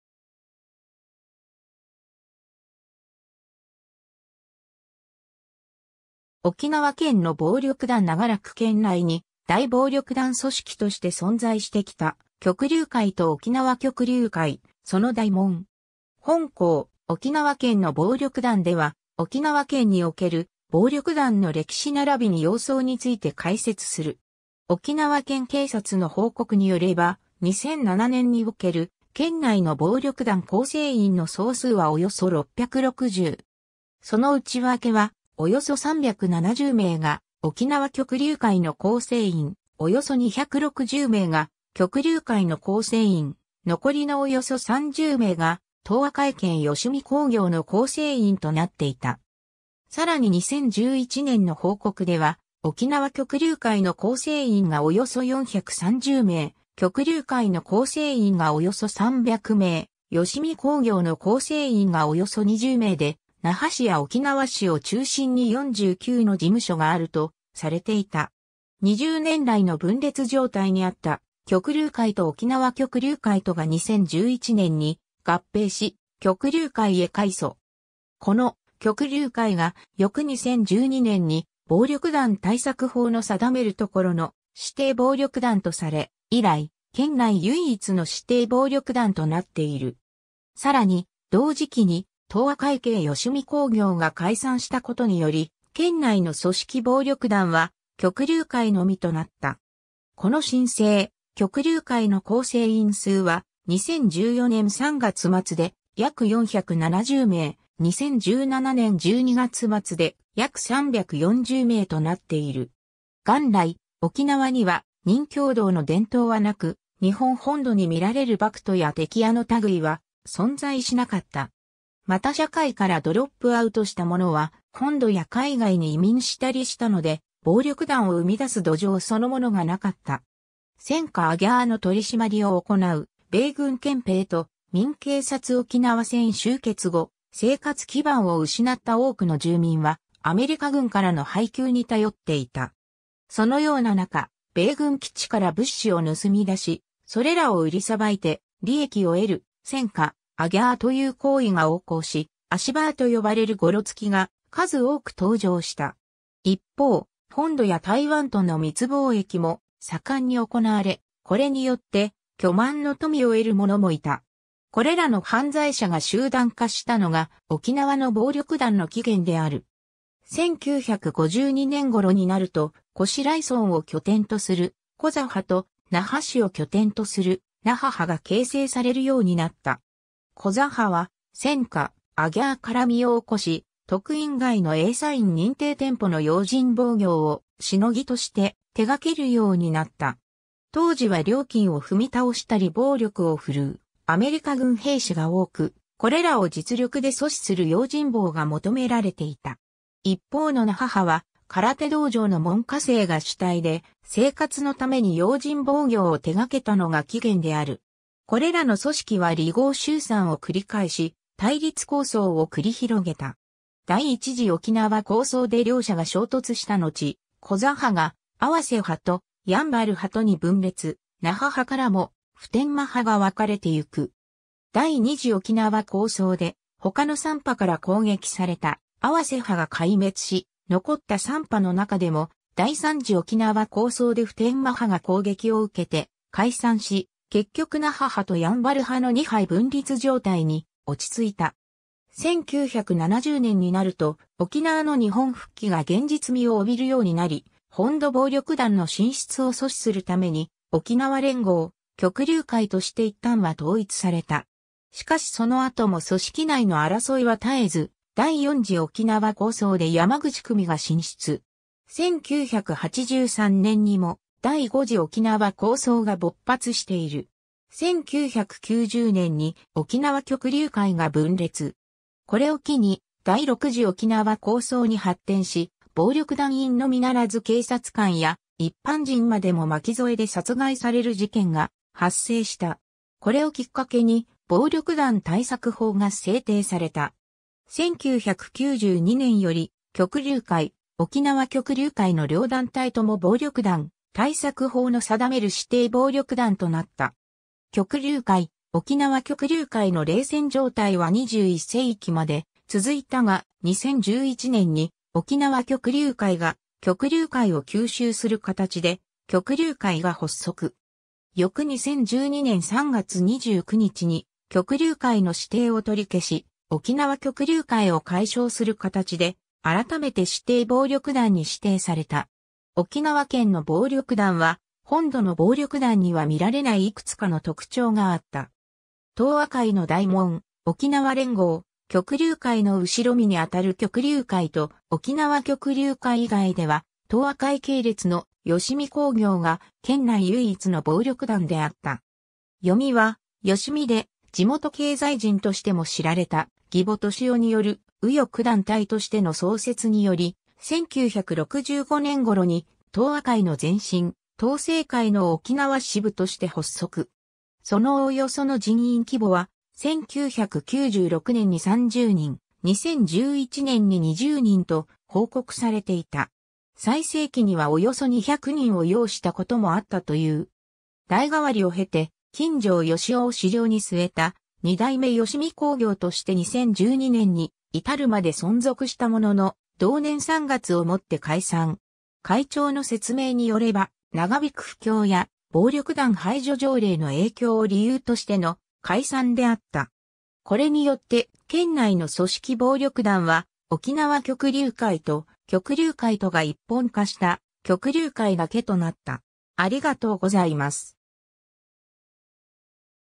沖縄県の暴力団
沖縄県の暴力団 長らく県内二大暴力団組織として存在してきた旭琉会（左）と沖縄旭琉会（右）、その代紋。
 本稿『沖縄県の暴力団』（おきなわけんのぼうりょくだん）では、沖縄県における暴力団の歴史ならびに様相について解説する。
 沖縄県警察の報告によれば、2007年における県内の暴力団構成員の総数はおよそ660。
その内訳は、およそ370名が沖縄旭琉会の構成員、およそ260名が旭琉会の構成員、残りのおよそ30名が東亜会系誼興業の構成員となっていた。
さらに2011年の報告では、沖縄旭琉会の構成員がおよそ430名、旭琉会の構成員がおよそ300名、誼興業の構成員がおよそ20名で、那覇市や沖縄市を中心に49の事務所があるとされていた。
 20年来の分裂状態にあった旭琉会と沖縄旭琉会とが2011年に合併し「旭琉會」へ改組。
この「旭琉會」が翌2012年に暴力団対策法の定めるところの指定暴力団とされ、以来、県内唯一の指定暴力団となっている。
さらに、同時期に東亜会系誼興業が解散したことにより、県内の組織暴力団は旭琉會のみとなった。
この新生「旭琉會」の構成員数は、2014年3月末で約470名、2017年12月末で約340名となっている。
 元来、沖縄には任侠道の伝統は無く、日本本土にみられる博徒や的屋の類は存在しなかった。
また社会からドロップアウトしたものは、本土や海外に移民したりしたので、暴力団を生み出す土壌そのものがなかった。
 『戦果アギャー』の取り締まりを行う米軍憲兵と民警察 沖縄戦終結後、生活基盤を失った多くの住民はアメリカ軍からの配給に頼っていた。
そのようななか、米軍基地から物資を盗み出し、それらを売り捌いて利益を得る「戦果アギャー」という行為が横行し、「アシバー」と呼ばれるゴロツキが数多く登場した。
一方、本土や台湾との密貿易も盛んに行われ、これによって巨万の富を得る者もいた。
これらの犯罪者が集団化したのが沖縄の暴力団の起源である。
 1952年頃になると、越来村（現沖縄市）を拠点とする『コザ派』と、那覇市を拠点とする『那覇派』が形成されるようになった。
コザ派は「戦果アギャー」から身を興し、特飲街のAサイン認定店舗の用心棒業を「シノギ」として手がけるようになった。
当時は料金を踏み倒したり暴力を振るうアメリカ軍兵士が多く、これらを実力で阻止する用心棒が求められていた。
一方の那覇派は空手道場の門下生が主体で、生活のために用心棒業を手がけたのが起源である。
 これらの組織は離合集散を繰り返し、対立抗争を繰り広げた。
第1次沖縄抗争（1961年～1962年）で両者が衝突したのち、コザ派が『泡瀬派』と『山原派』とに分裂、那覇派からも『普天間派』が分かれてゆく。
第2次沖縄抗争（1964年～1966年）で他の3派から攻撃された泡瀬派が壊滅し、残った3派の中でも第3次沖縄抗争（1967年）で普天間派が攻撃を受けて解散し、結局那覇派と山原派の二派分立状態に落ち着いた。
 1970年になると沖縄の日本復帰が現実味を帯びるようになり、本土暴力団の進出を阻止するために『沖縄連合旭琉会』（のちの旭琉会）として一旦は統一された。
しかしその後も組織内の争いは絶えず、第4次沖縄抗争（1974年～1981年）で山口組が進出。
1983年にも第5次沖縄抗争が勃発している。
 1990年に沖縄旭琉会が分裂。
これを機に第6次沖縄抗争（1990年～1992年）に発展し、暴力団員のみならず警察官や一般人までも巻き添えで殺害される事件が発生した。
これをきっかけに暴力団対策法が制定された。
 1992年より旭琉会・沖縄旭琉会の両団体とも暴力団対策法の定める指定暴力団となった。
 旭琉会・沖縄旭琉会の冷戦状態は21世紀まで続いたが、2011年に沖縄旭琉会が旭琉会を吸収する形で『旭琉會』が発足。
翌2012年3月29日に旭琉会の指定を取り消し、沖縄旭琉会を改称する形で改めて指定暴力団に指定された。
 沖縄県の暴力団は本土の暴力団には見られない幾つかの特徴があった。
 東亜会の代紋 『沖縄連合旭琉会』の後身にあたる旭琉会と沖縄旭琉会以外では、東亜会系列の『誼興業』が県内唯一の暴力団であった。
読みは“よしみ”（こうぎょう）で、地元経済人としても知られた宜保俊夫による右翼団体としての創設により、1965年頃に東亜会の前身『東声会』の沖縄支部として発足。
そのおおよその人員規模は、1996年に30人、2011年に20人と報告されていた。
最盛期にはおよそ200人を擁したこともあったという...